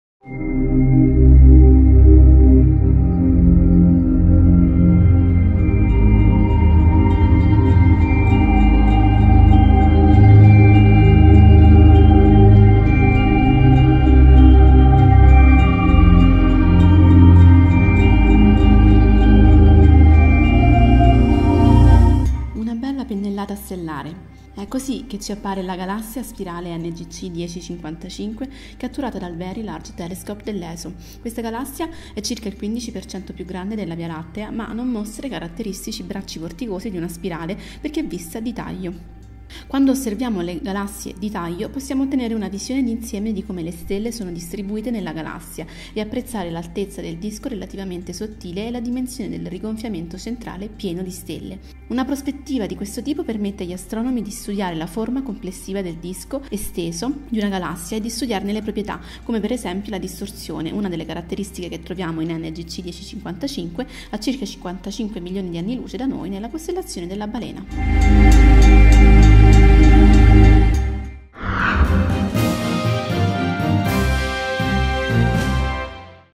Una pennellata di stelle e gas per il VLT dell'ESO
Una bella pennellata stellare. E' così che ci appare la galassia a spirale NGC 1055 catturata dal Very Large Telescope dell'ESO. Questa galassia è circa il 15% più grande della Via Lattea ma non mostra i caratteristici bracci vorticosi di una spirale, perchè è vista di taglio. Quando osserviamo le galassie di taglio, possiamo ottenere una visione d'insieme di come le stelle sono distribuite nella galassia, e apprezzare l'altezza del disco relativamente sottile e la dimensione del rigonfiamento centrale, pieno di stelle. Una prospettiva di questo tipo permette agli astronomi di studiare la forma complessiva del disco esteso di una galassia e di studiarne le proprietà, come per esempio la distorsione, una delle caratteristiche che troviamo in NGC 1055, a circa 55 milioni di anni luce da noi nella costellazione della Balena.